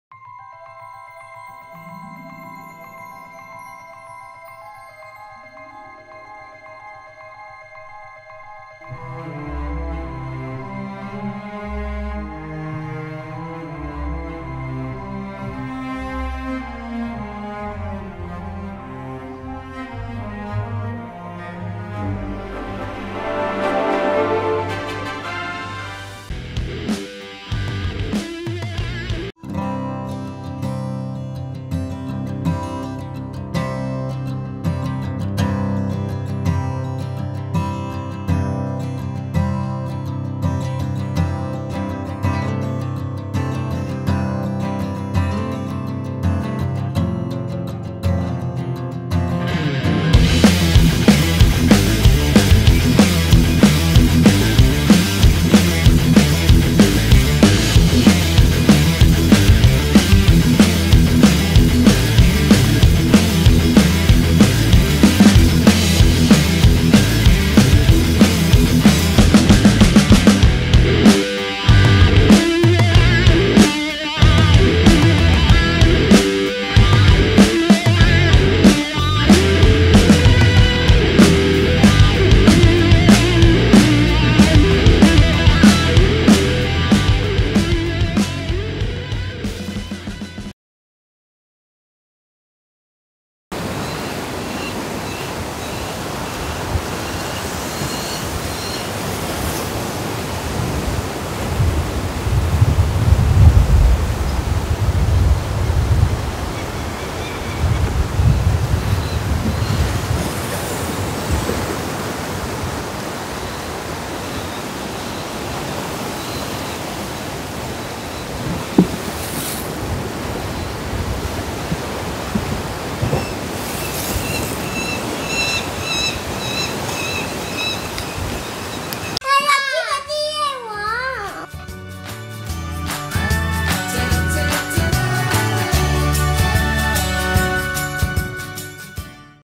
(特別版)_酋長岩遊隼_SP10_幼鳥對爪_Falco peregrinus_2023年5月20日
2023年5月20日酋長岩遊隼幼鳥對爪
SONY A7S3  4K120 , SONY 200-600mm